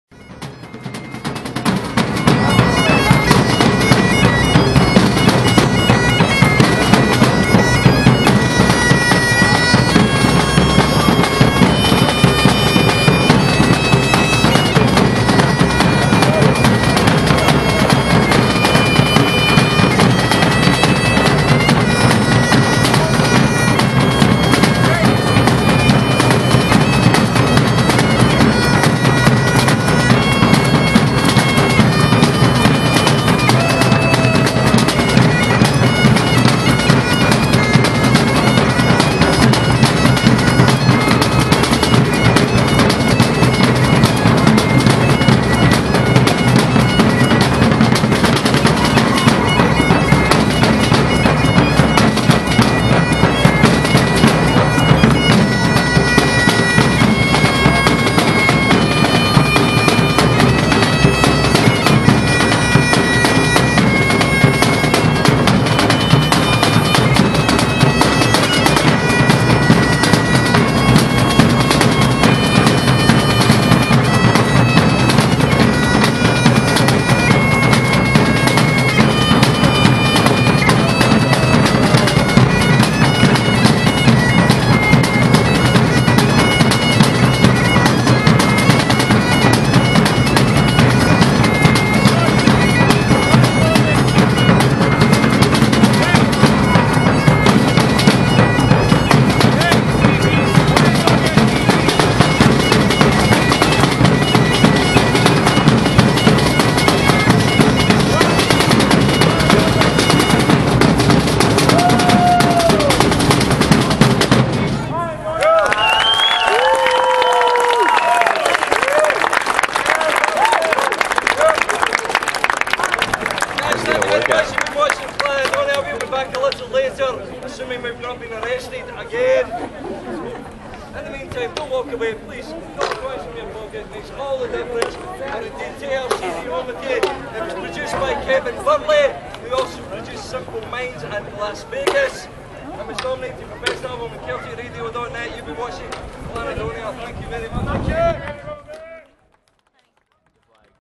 Scottish Drummers - Edinburgh, Scotland - May 2009
Fun times on our first international trip together!